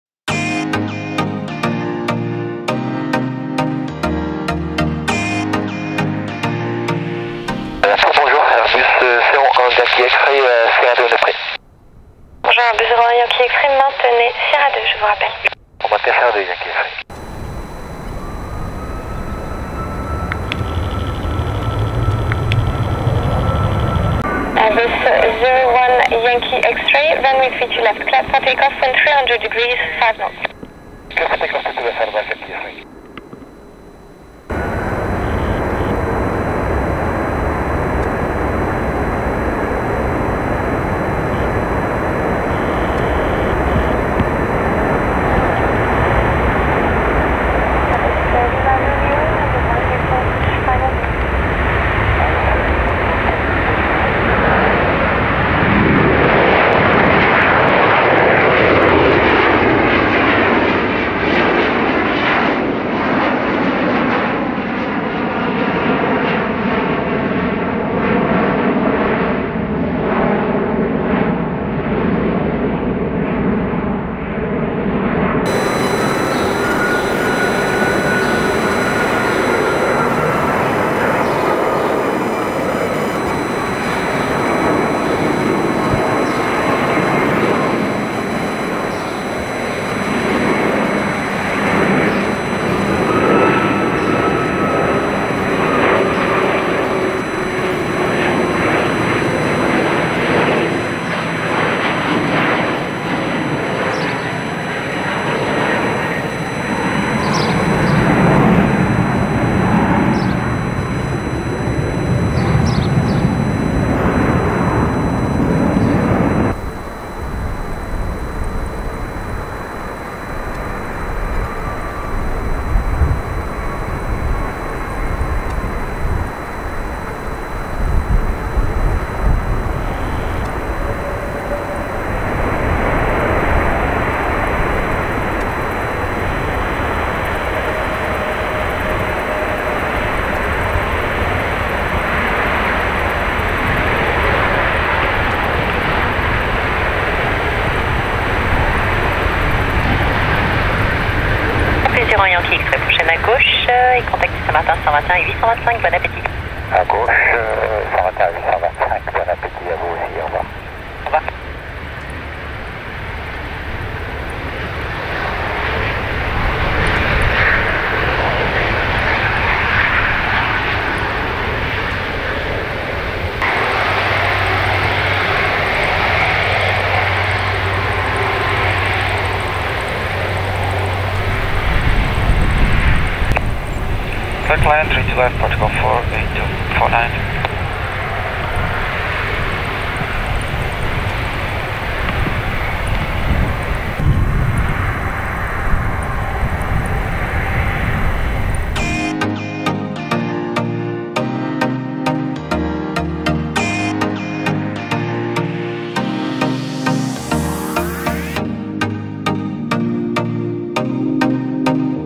The 6th and Final AIRBUS BELUGA XL6 | First Flight | Toulouse Airbus Factory
6th and Final Airbus Beluga XL - First Flight - Special Livery - July 21th 2023 - Toulouse